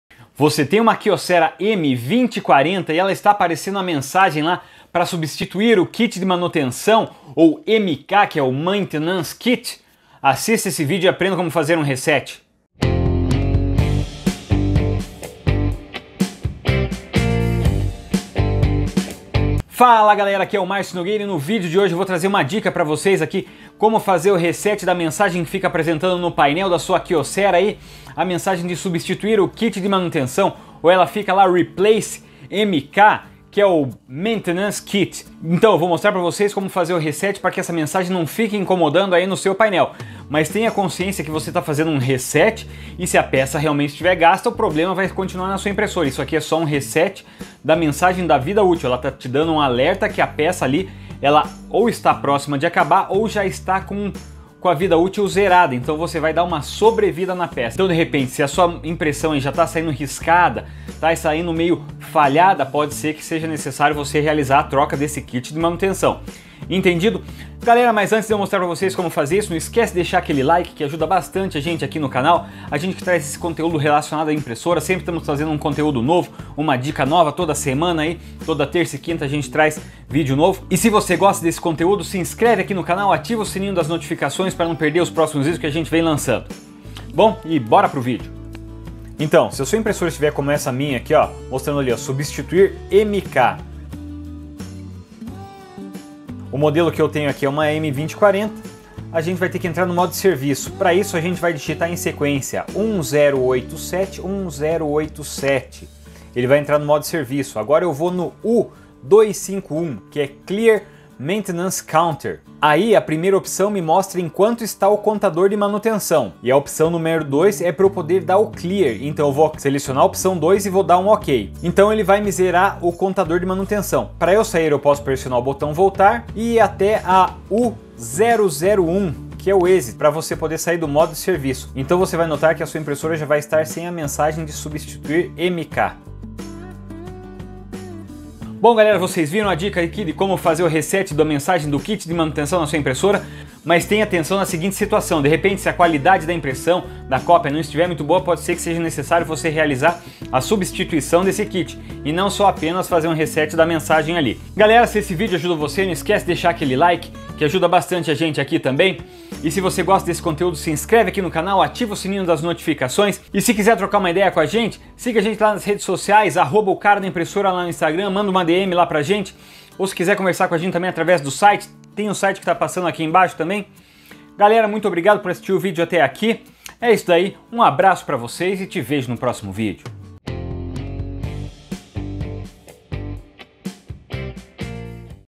Como fazer reset do kit de manutenção - Kyocera M2040
O Cara da Impressora

No vídeo de hoje mostro como realizar o reset da mensagem "Substituir MK" no modelo M2040 da Kyocera.

Faça uma parceria com o Canal, nos envie uma mensagem, pois podemos produzir vídeos que auxiliem os usuários e também os técnicos.